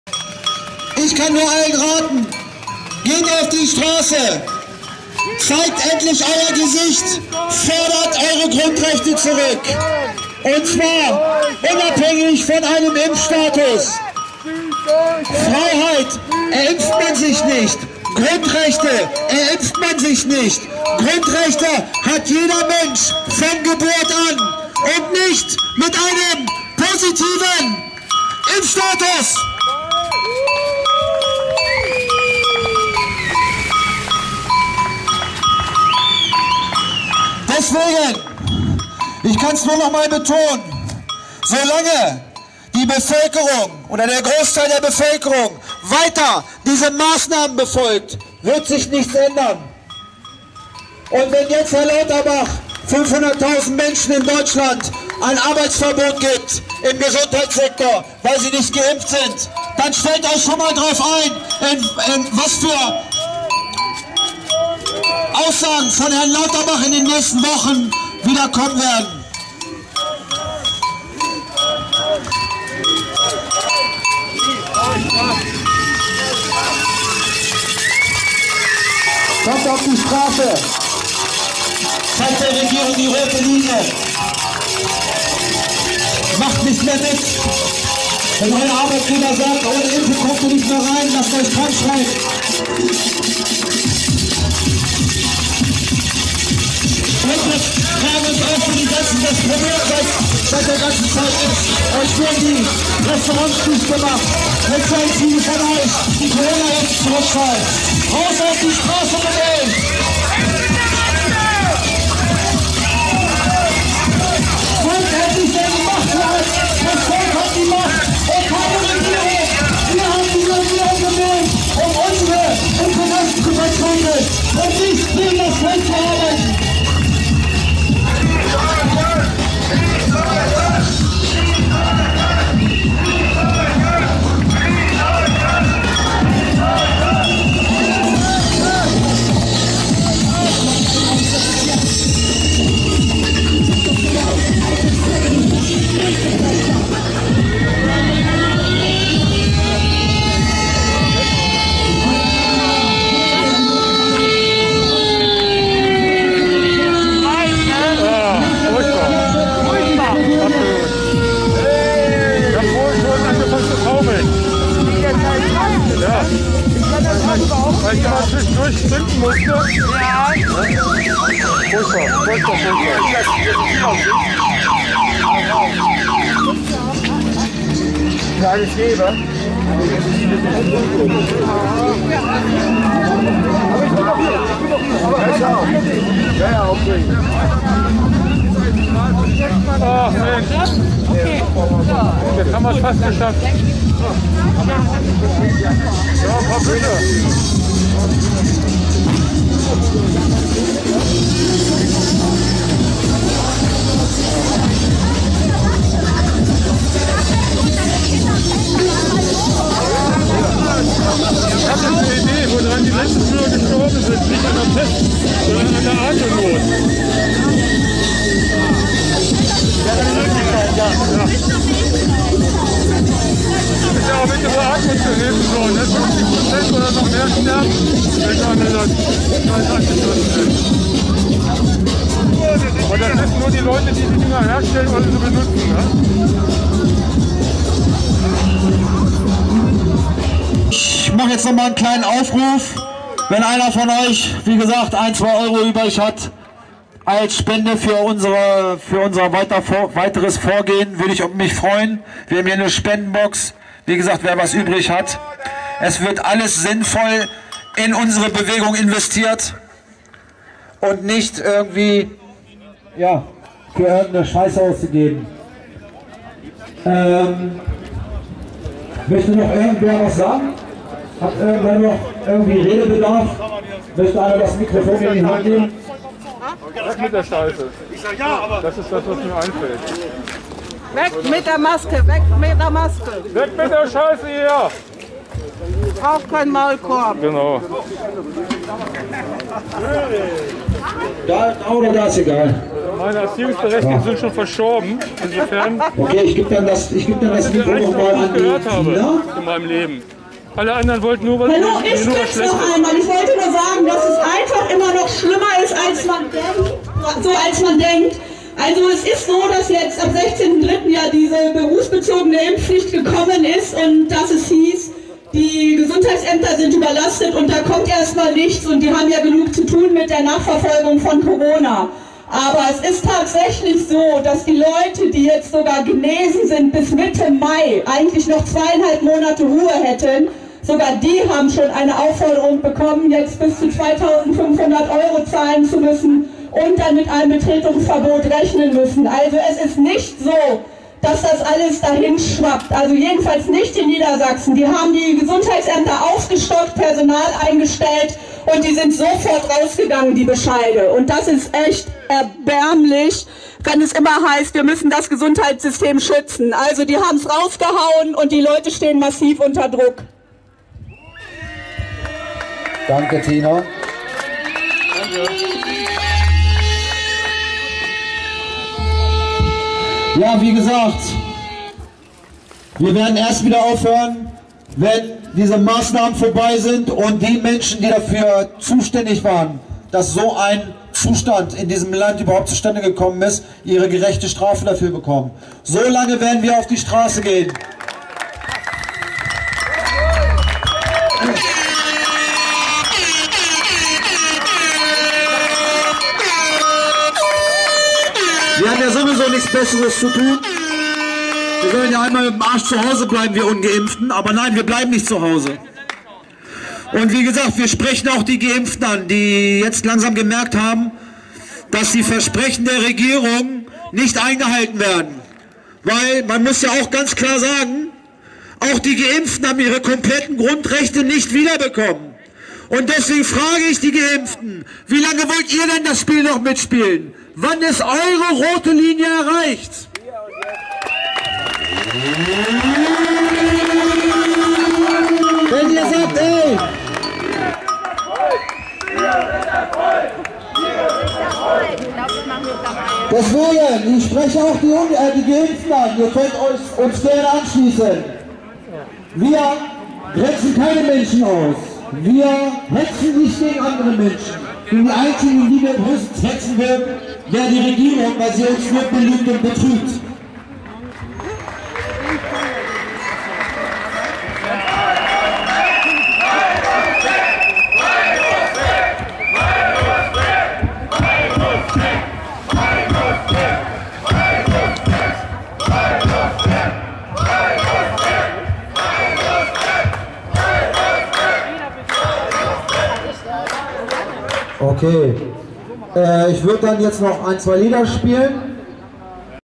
23.03.2022 Hannover Ricklingen. Die Mittwochsdemo Walk to Freedom Teil 10
Die letzten Meter der Demo mit Abschluss Kundgebung. ab Min:5:00 wird es Interessant.